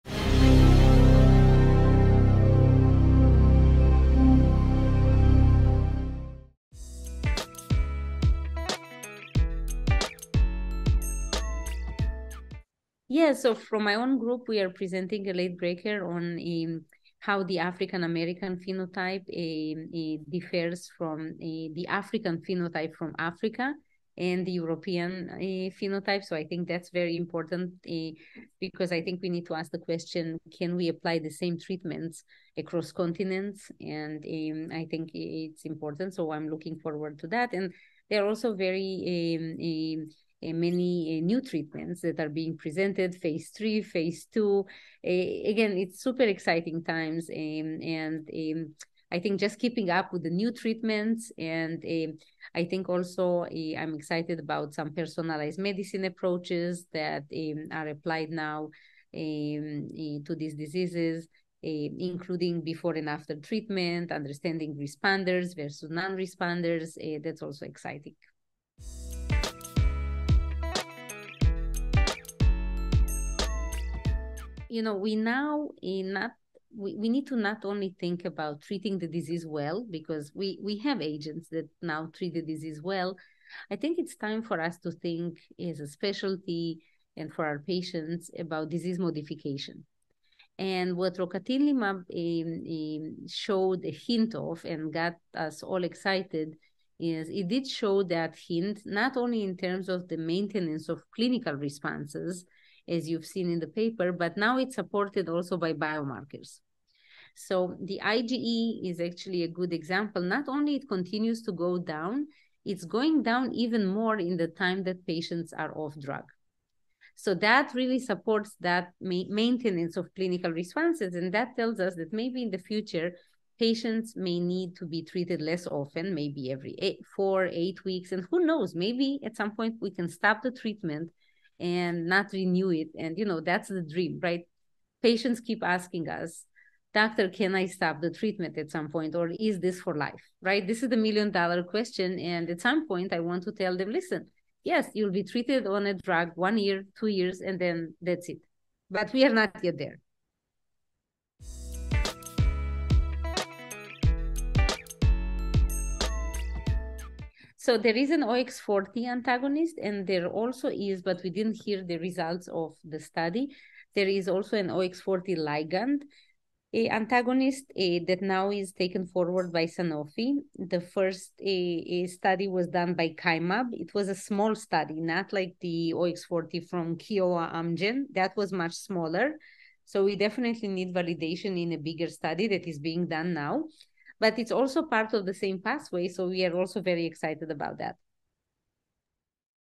Emma Guttman-Yassky of Icahn School of Medicine Talks Developments in Atopic Dermatitis Treatment
Emma Guttman-Yassky, M.D., Ph.D. of Icahn School of Medicine at Mount Sinai, New York discussed developments in atopic dermatitis treatment, more specifically with therapies like rocatinlimab and other anti-OX4 agents today at the annual American Academy of Dermatology meeting in New Orleans.